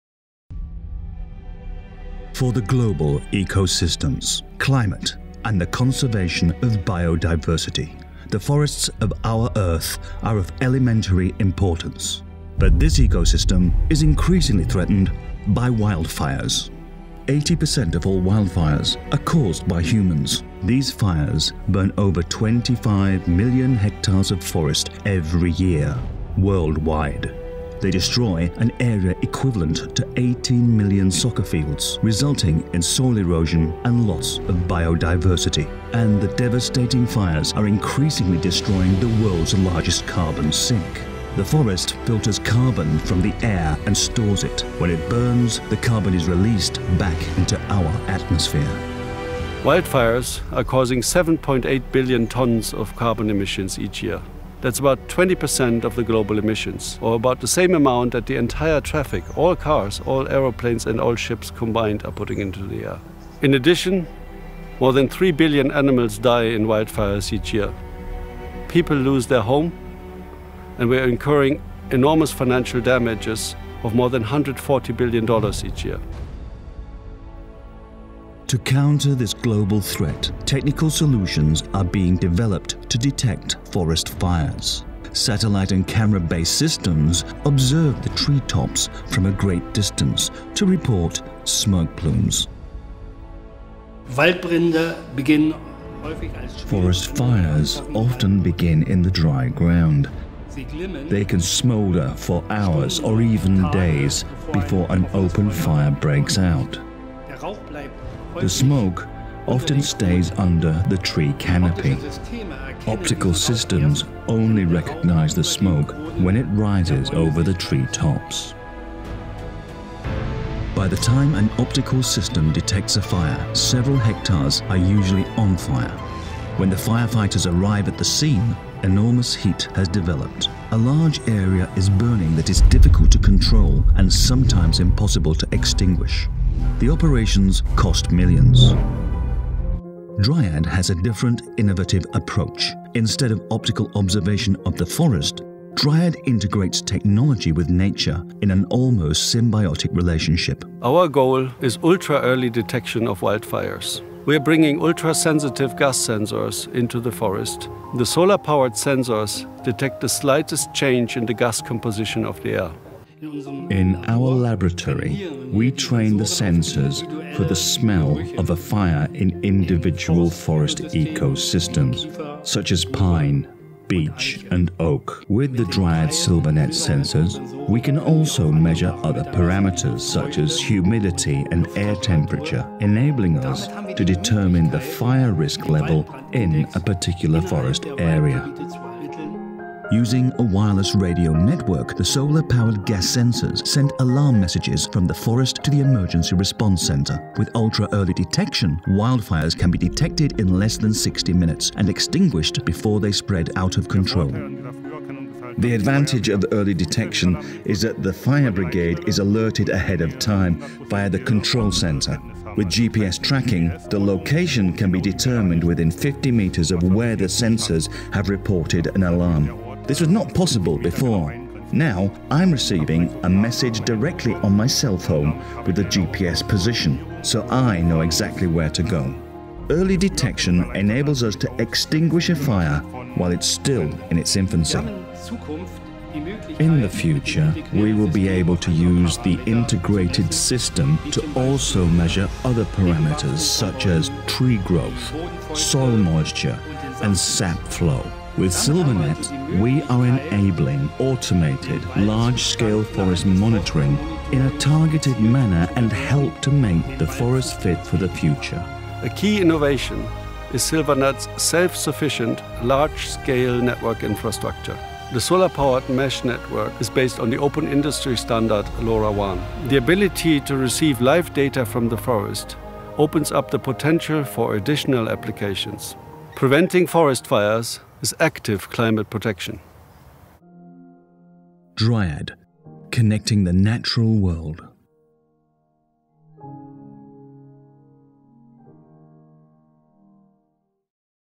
Dryad Company Video (5min)
Introduction to Dryad's Silvanet, providing ultra-early detection for wildfires using a large-scale, solar-powered IoT sensor network placed in the forest.